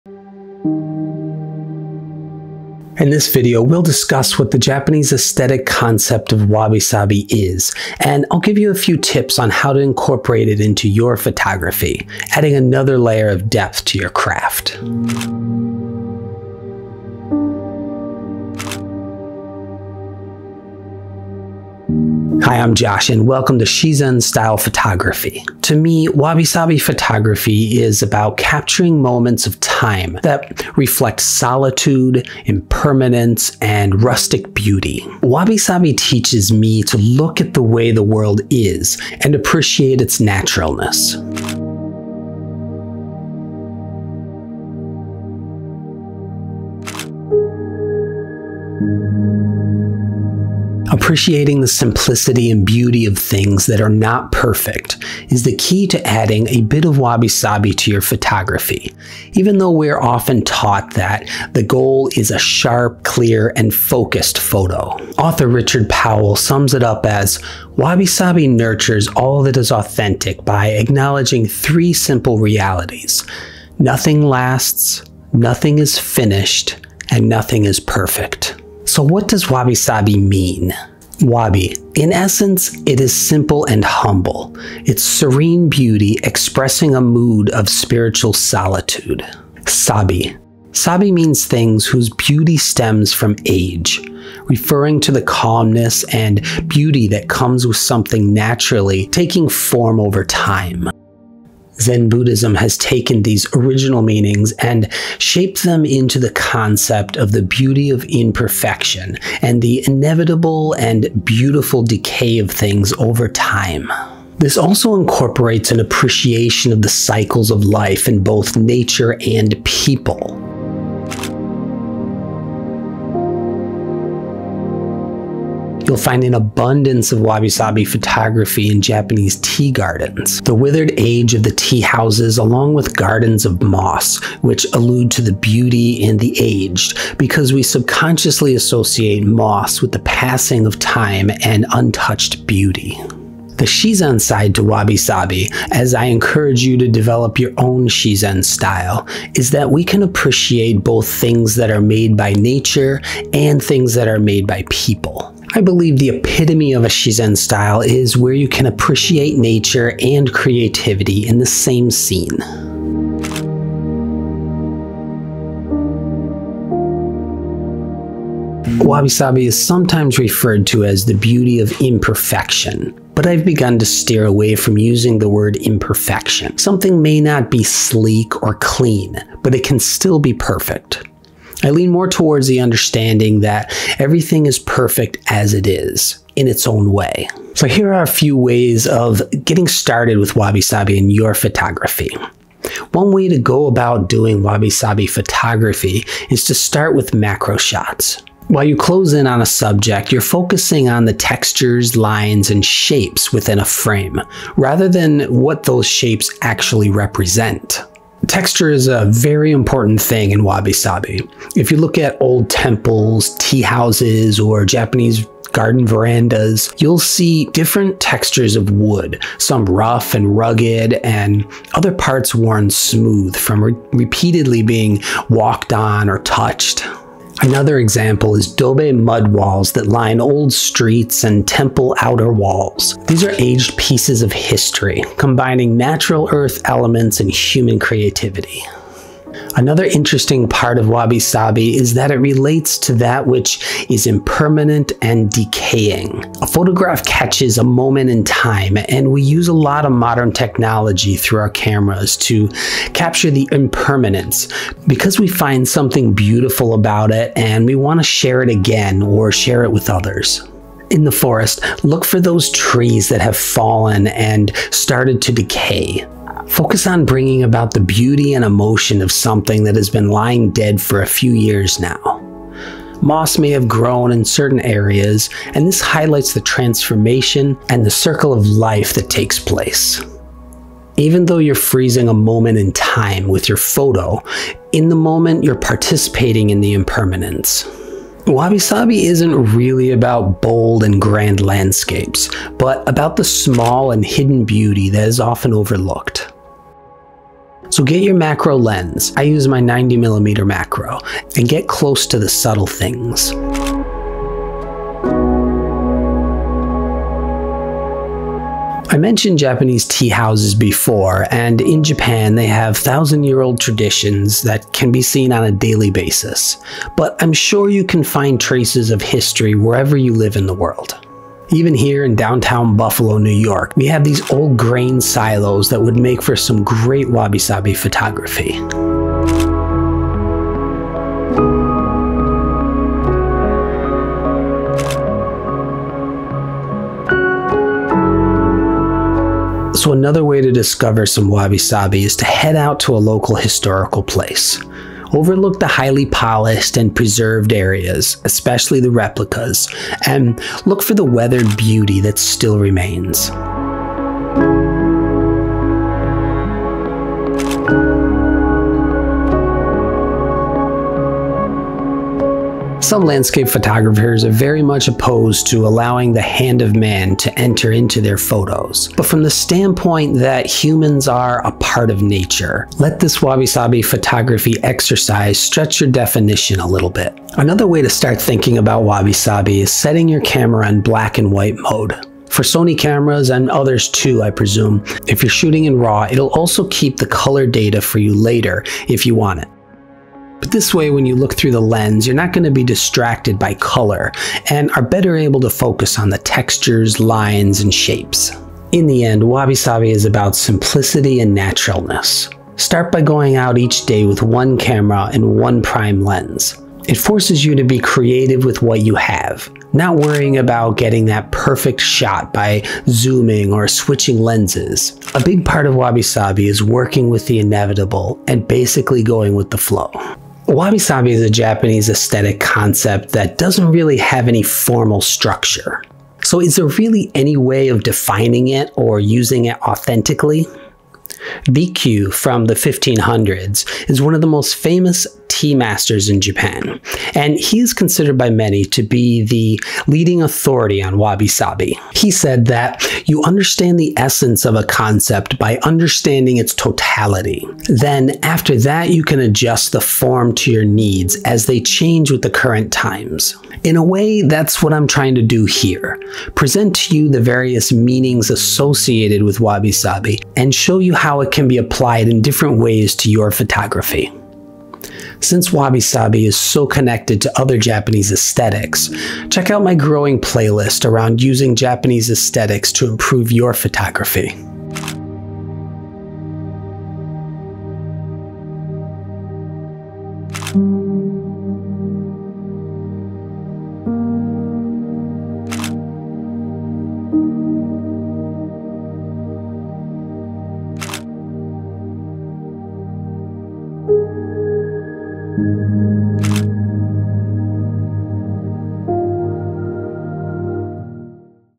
Wabi-Sabi Photography: Tips for Finding Beauty in the Imperfect and Impermanent
In this video, we’ll discuss what the Japanese aesthetic concept of wabi-sabi is and I’ll give you a few tips on how to incorporate it into your photography, adding another layer of depth to your craft...

◦ Visit Shizen Style for learning resources like E-books, my blog, newsletter, shopping etc.